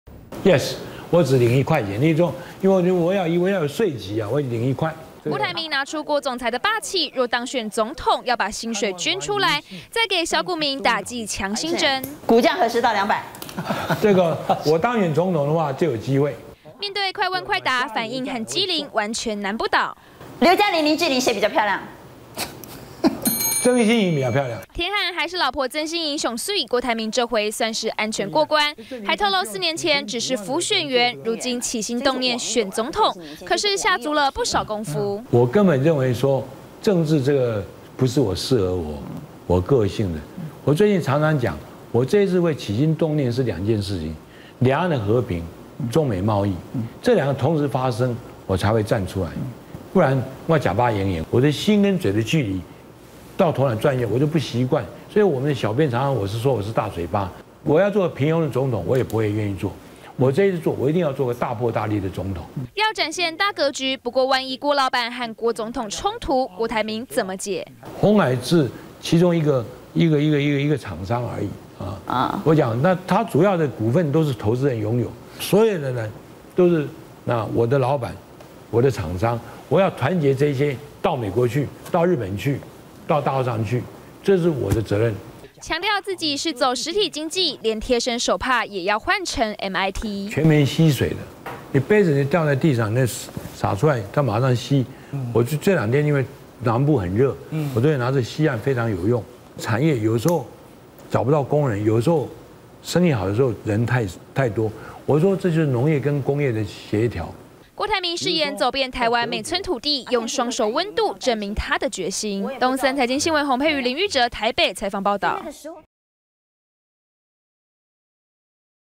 陳斐娟獨家專訪郭台銘 快問快答郭董機智反應
國民黨總統初選參選人郭台銘，今天接受東森財經台主持人陳斐娟的專訪，長達三個小時的有問必答，尤其鴻海股東最想知道 沒有郭台銘的鴻海該怎麼辦，郭董也首度作出回應！